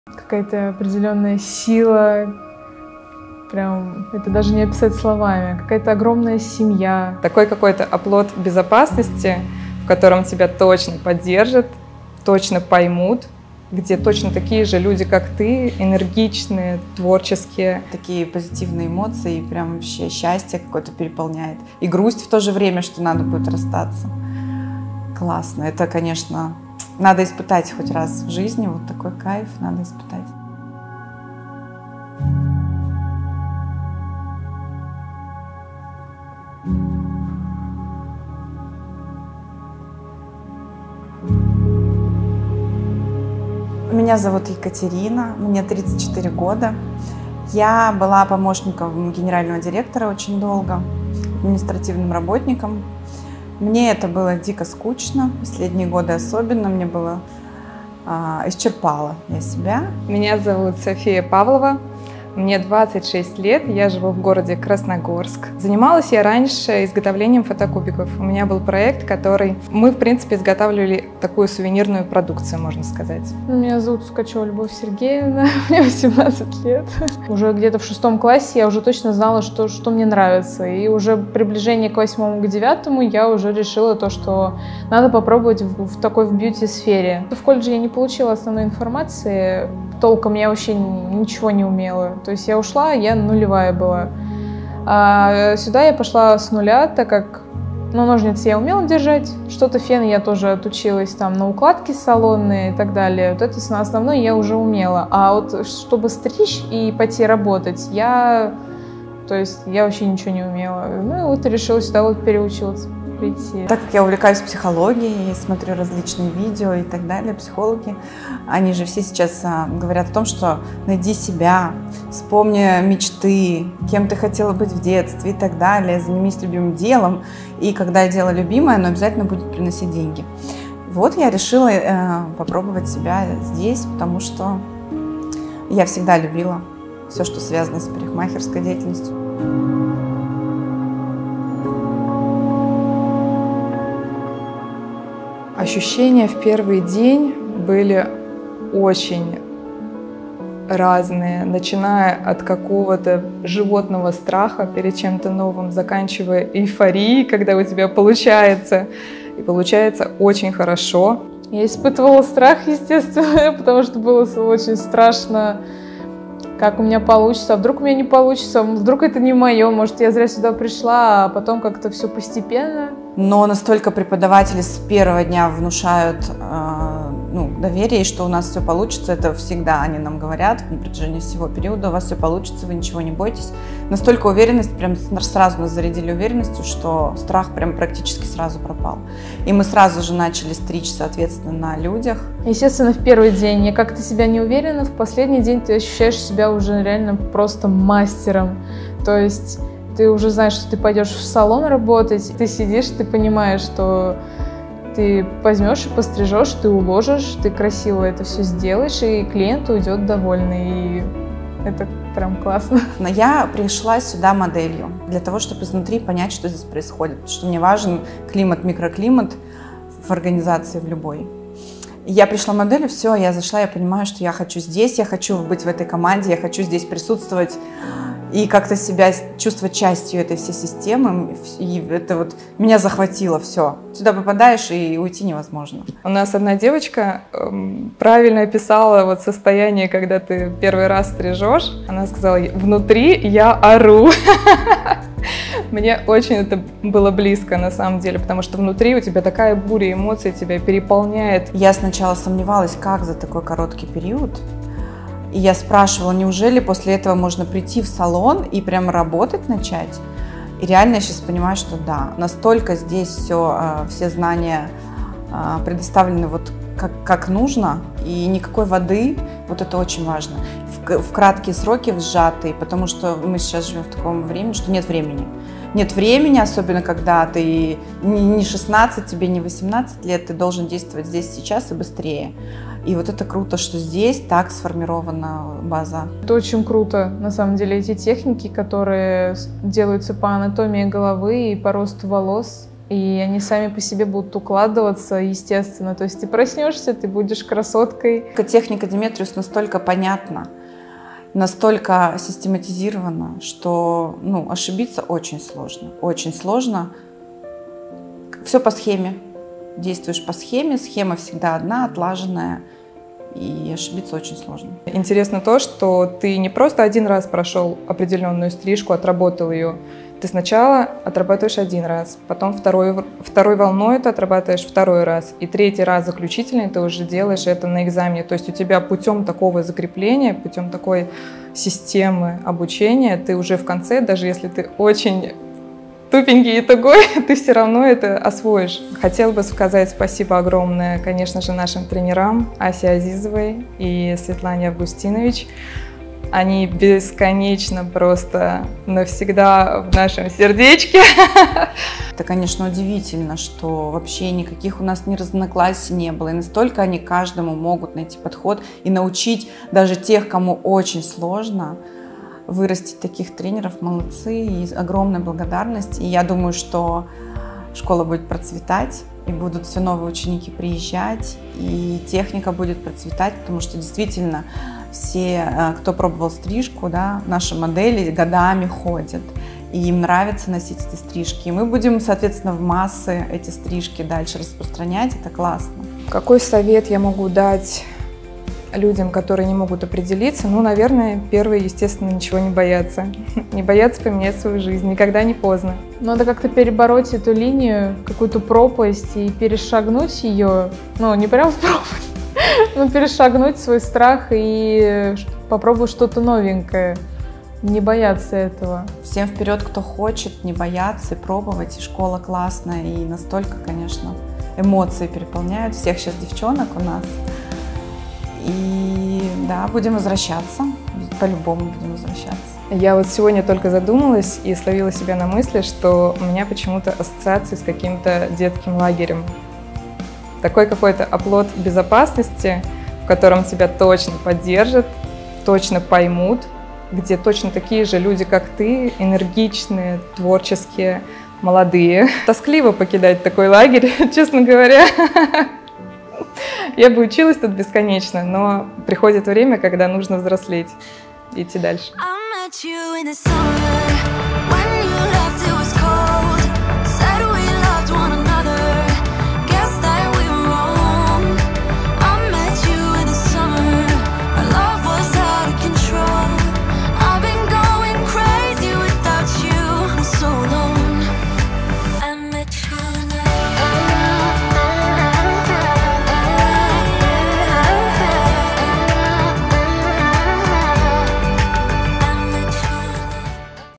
"Такие разные, но мечта одна..." / Отзыв об обучении с нуля в школе Деметриус / Отзывы о Demetrius
Отзывы учеников о школе - это самая приятная часть нашей работы. Скрывать не будем, больше самого обучения мы, как школа Деметриус, любим видеть эту настоящую радость, настоящее вдохновение, настоящую целеустремленность и уверенность в себе, в своих силах, на лицах наших выпускников.
В этот раз своими впечатлениями поделились выпускники обучения с нуля школы Деметриус!
Мы желаем вам успехов в работе, преданных клиентов, никогда не останавливаться, даже если вас будут пытаться остановить, и идти к своим намеченным целям!

А для тех, кто ещё сомневается идти ли на обучение, куда идти и вообще стоит ли, мы сняли это видео!

DEMETRIUS Школа концептуальной стрижки (с)
Узнать и записаться на обучение: